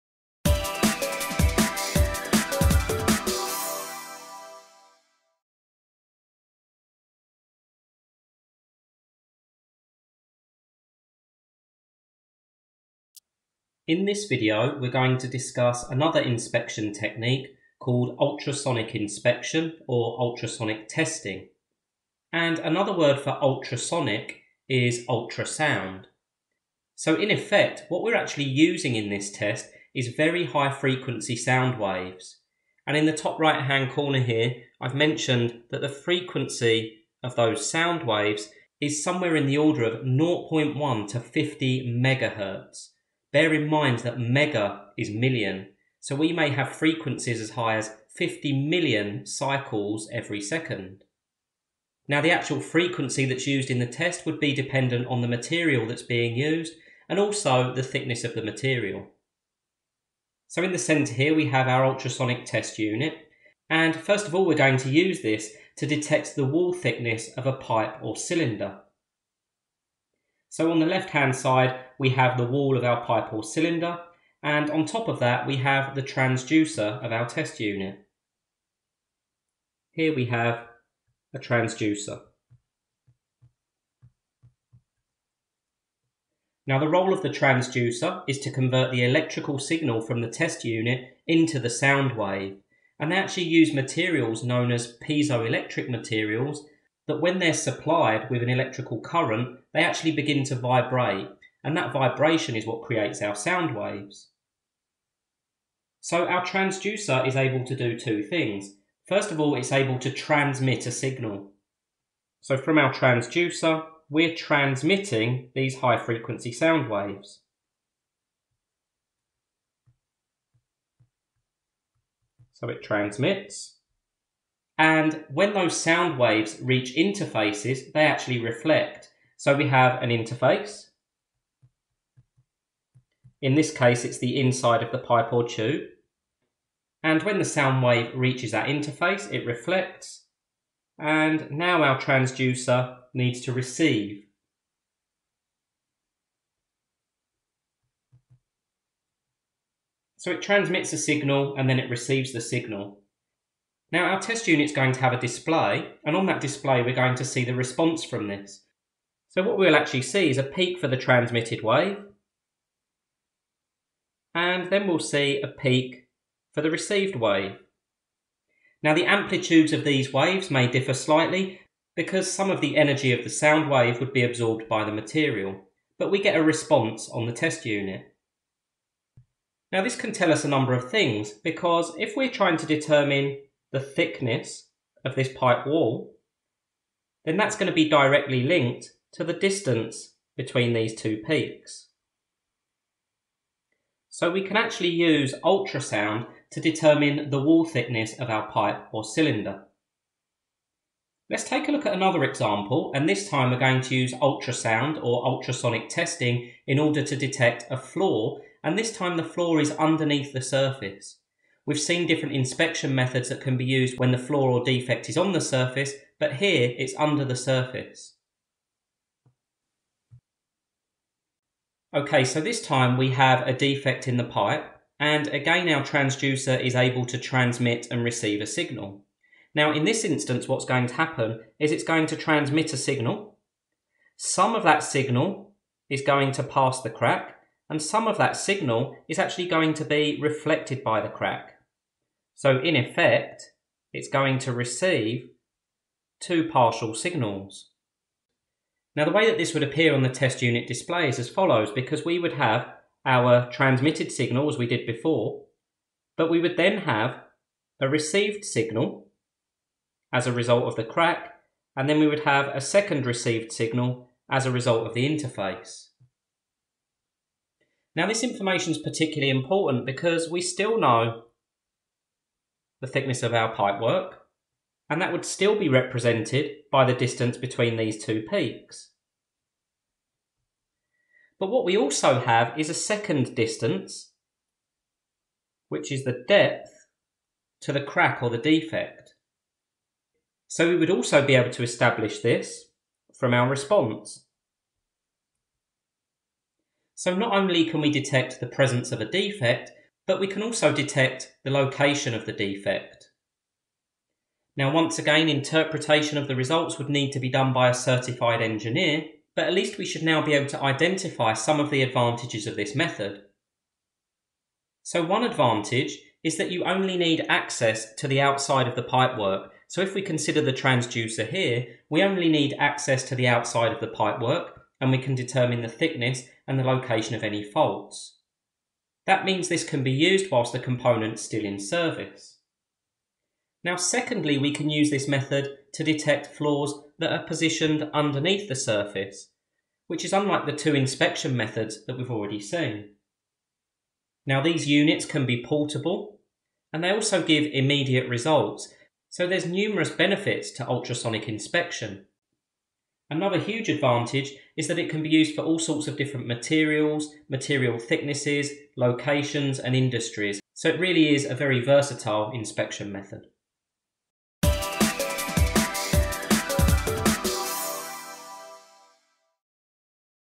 Ultrasonic Inspection for Identifying Wall Thicknesses and Sub Surface Defects
This video outlines the non-destructive testing method of Ultrasonic inspection, where high frequency sound waves ('ultra sound') are used to detect wall thicknesses, as well as defects under the surface of components.